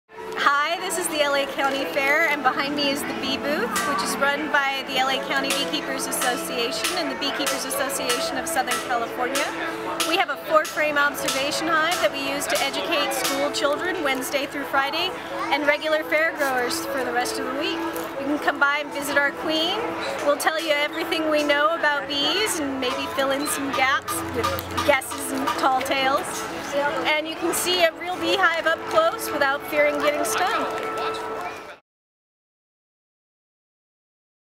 L.A. County Fair 2013 - Bee Exhibit
Learn about the bees and try some flavored honey at the Bee Exhibit!